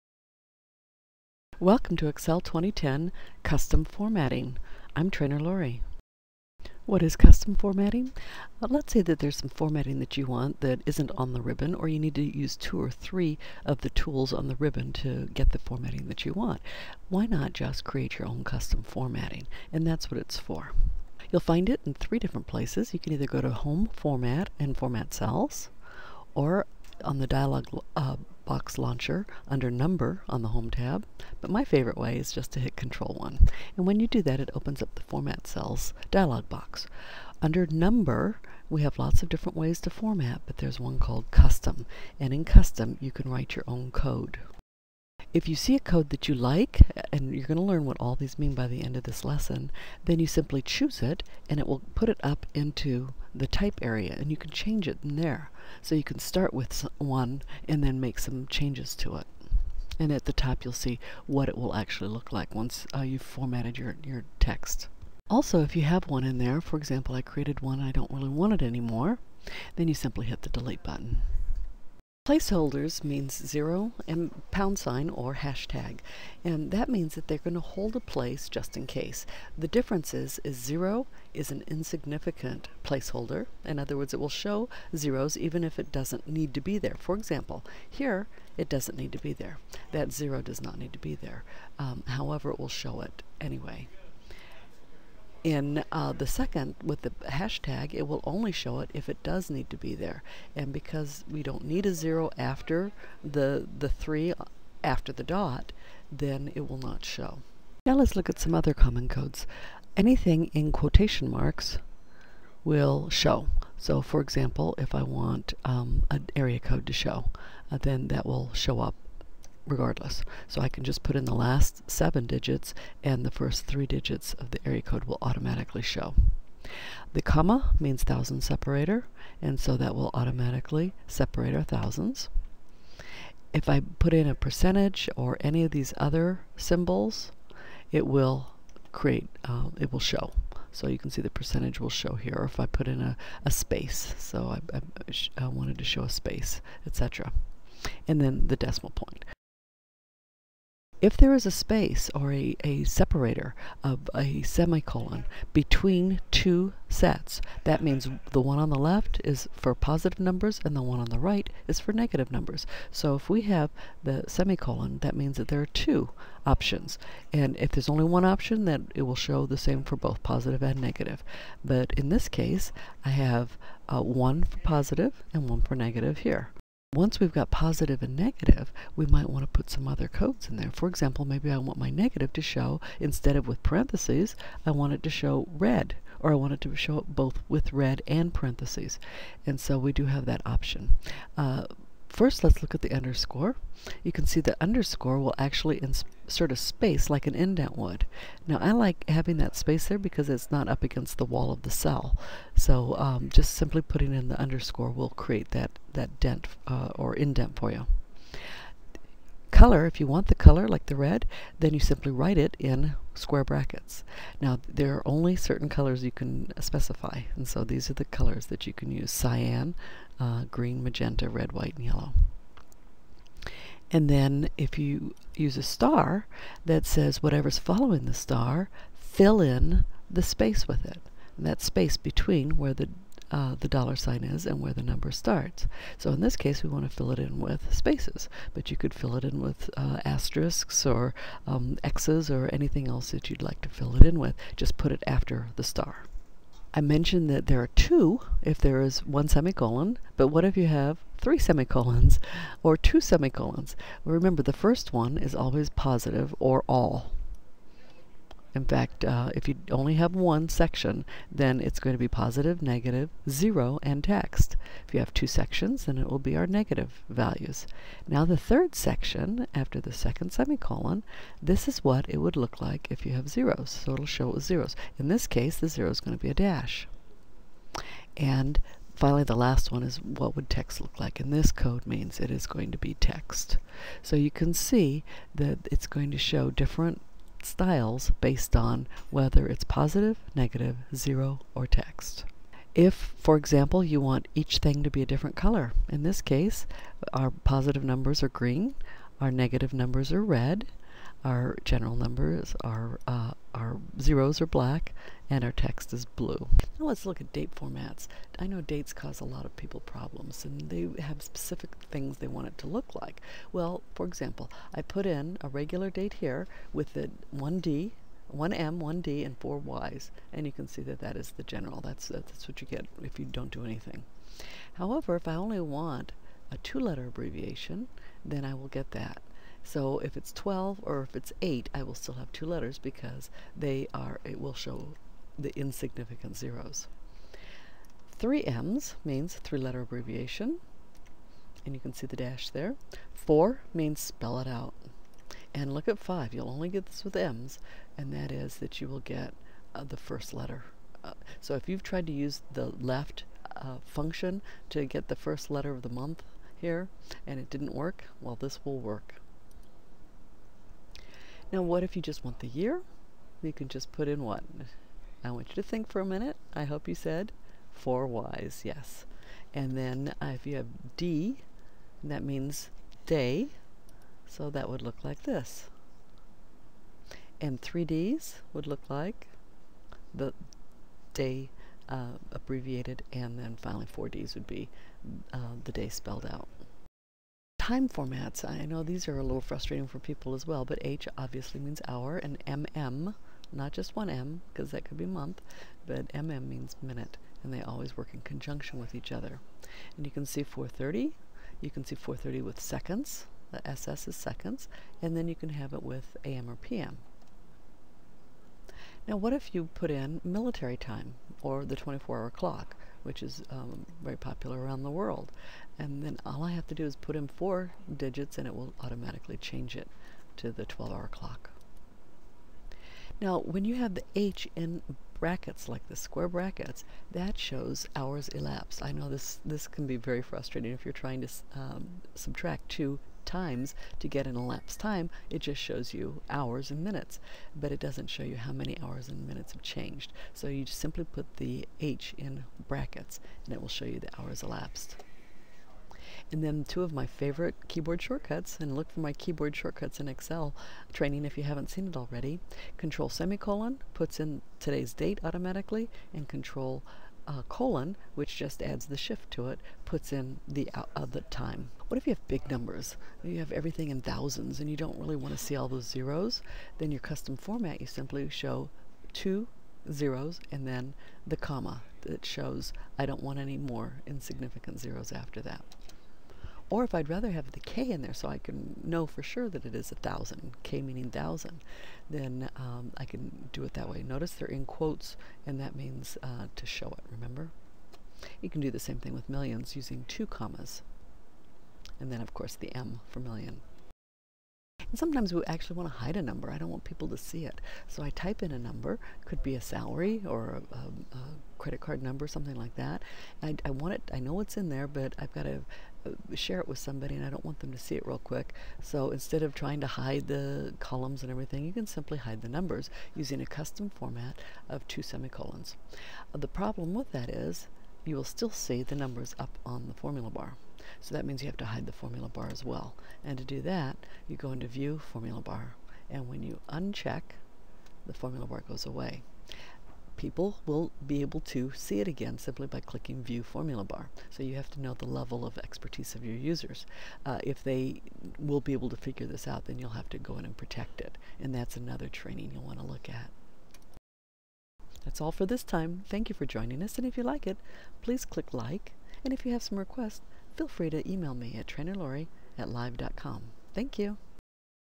Excel - Custom Formatting
See how to create your own number and date formatting in Excel to make it look exactly how you want!